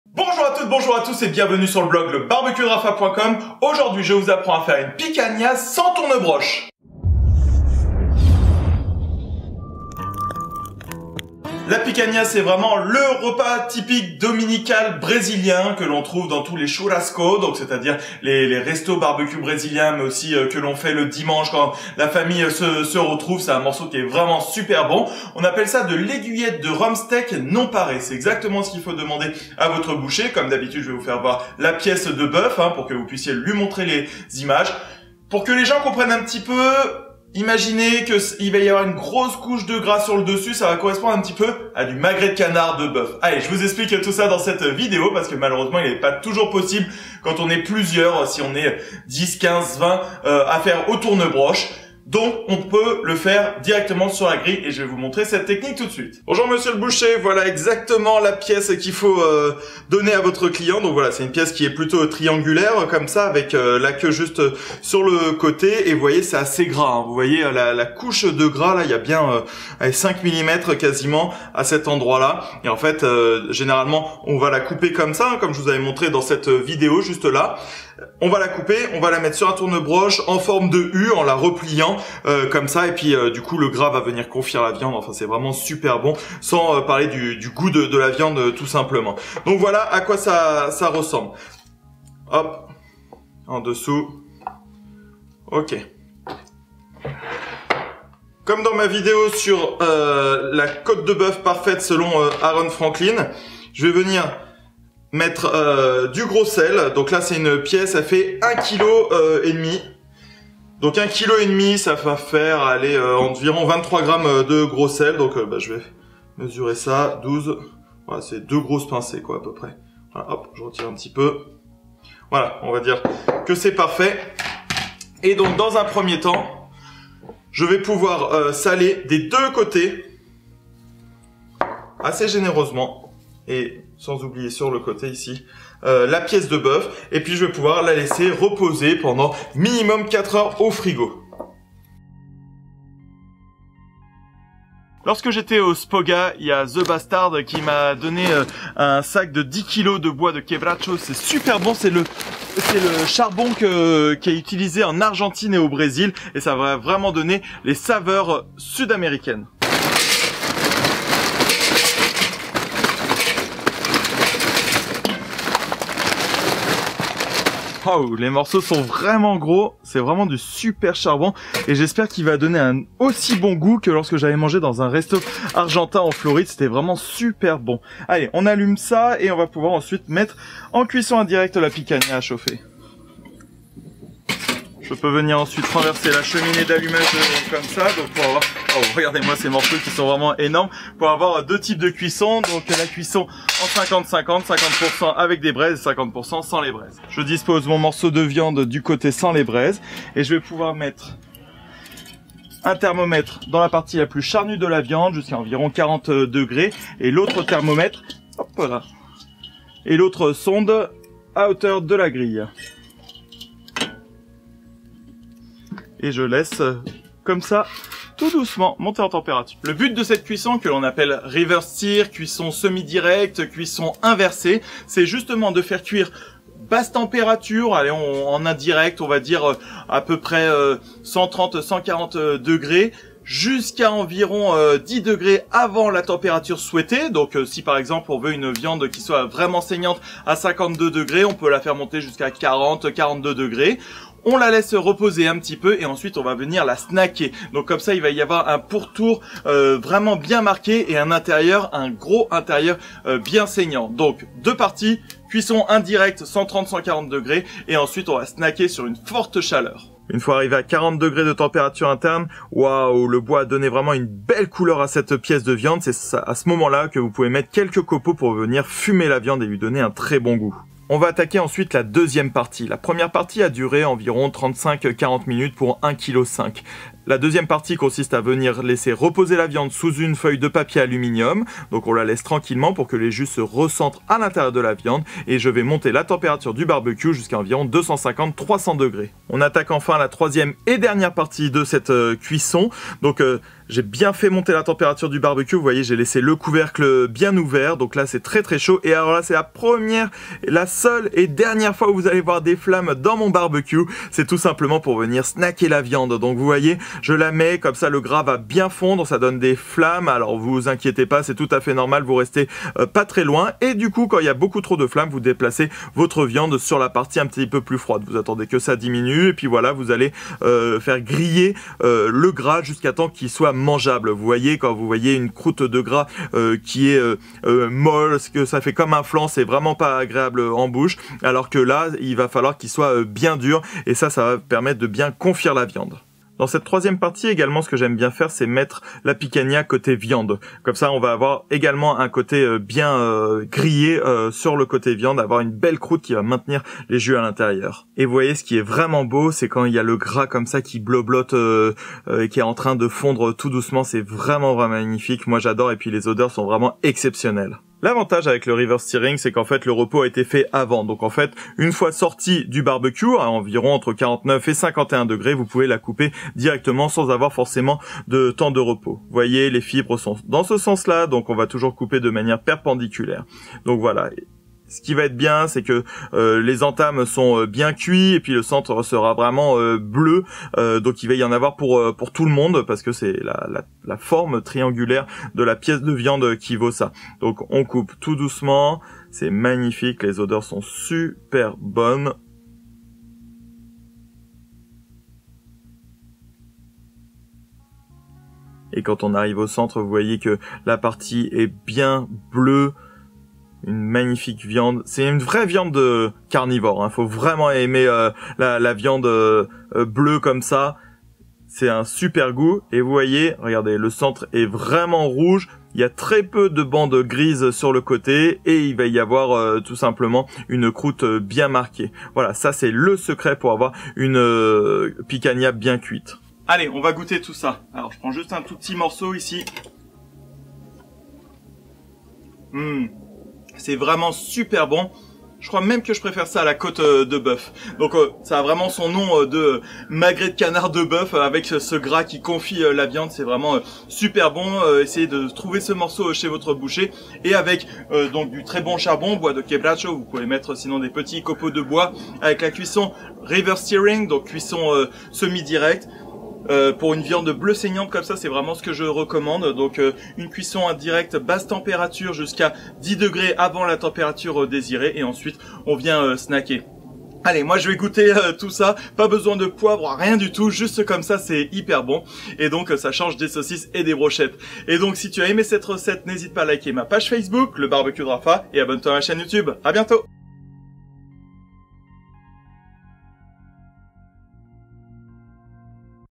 Comment faire cuire une Picanha sans tourne broche ?
La Picanha est une vraie viande de viandard a tester absolument ! Demander a votre boucher une aiguillette de rumsteak non parée, avec un bon charbon et une cuisson parfaite, vous aurez une trés jolie viande super savoureuse !
Il ne vous reste plus qu'à la trancher en fines tranches et à la servir avec de la sauce chimichurri par exemple :)